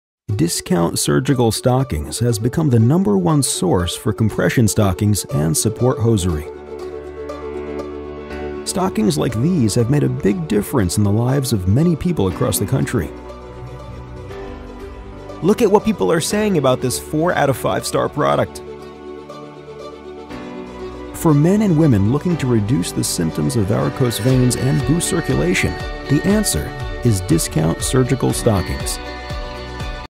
Juzo 4202 A-D Short Length Ribbed Knee-Hi 30-40mmHg - Khaki
Why compromise? With classic ribbed styling and the benefits of compression therapy, you can have the additional support you need and the wardrobe versatility you want - whether youre in an all-day business meeting or taking an all-day bus trip!Classic ribbed style Smooth, flat, stay-put band Reinforced heel and toe Comfortable Top band to keep stockings in place

Juzo 4202 A-D Short Length Ribbed Knee-Hi 30-40mmHg - Khaki Size II